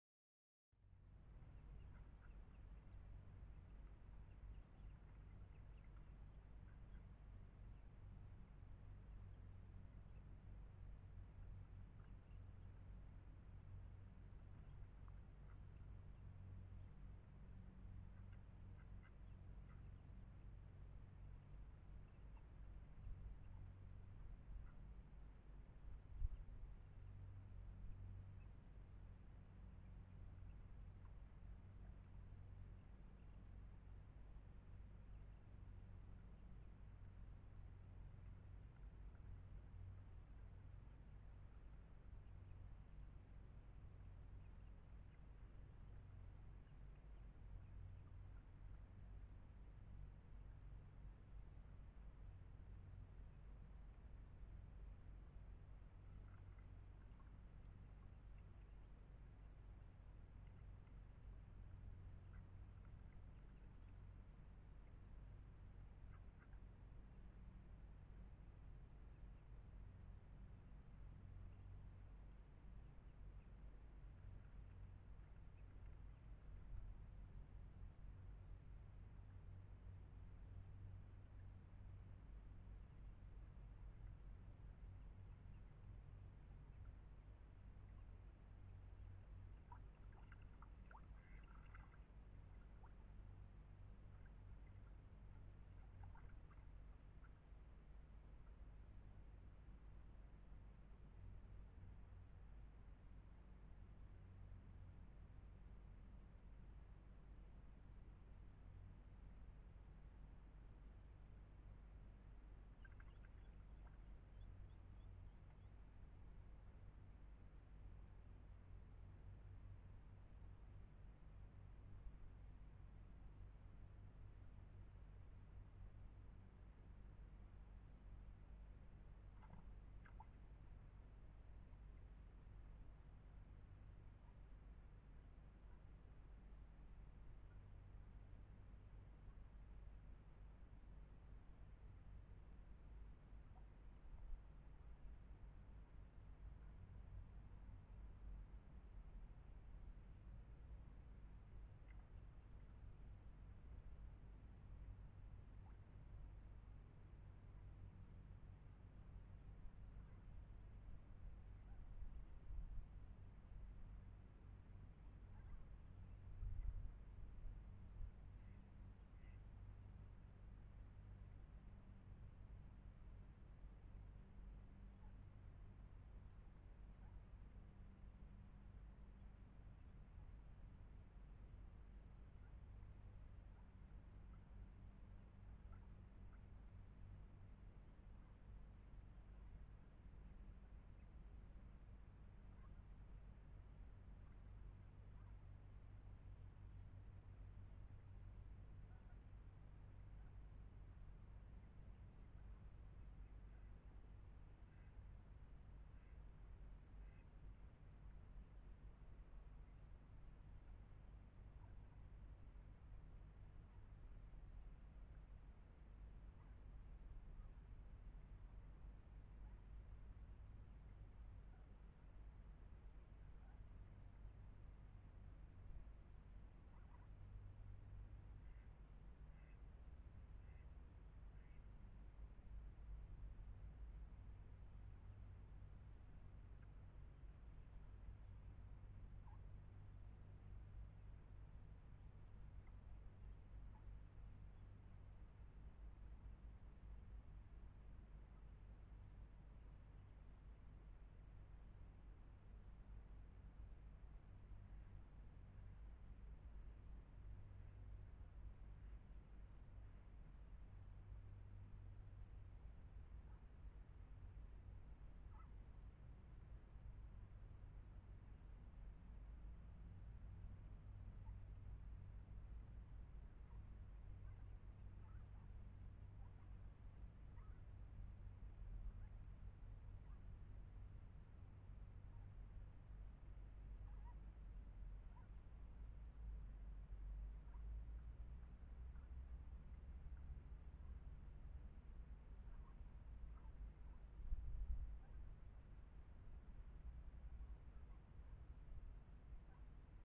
Entenküken Update, Tag 9: Die Flauschigen Fünf und der Spätzünder
Friede, Freude, Harmonie: Wenn's doch immer so zuginge wie früh morgens. Die Idylle täuscht aber: Nur fünf von elf Küken haben die erste Woche überlebt.

Jetzt sind wir in Woche 2 und hoffen, dass der natürlichen Auslese damit Genüge getan wurde. No more Verluste, please... Aber keine Sorge, hier gibt's keinen "natürliche Auslese-Horror", nur flauschige Wohltu-Filmchen. Viel Spaß! (c:

Unter den Flauschigen Fünf ist immer mal einen Einzelgänger, der entweder besonders mutig ist oder ein Träumerle, das sich gedankenverloren vom Rest der Bande trennt. So genau wissen wir das nicht. Wir sind uns nichtmal sicher, dass es jedesmal der oder die selbe ist, sehn ja alle gleich niedlich aus, noch.

Auf diesem Meisterwerk der Tierdokumentation ist das mal filmisch festgehalten. Ein bisschen Geduld müsst ihr allerdings mitbringen, also nicht vorspulen, wenn mal en Weile lang nix passiert! (c;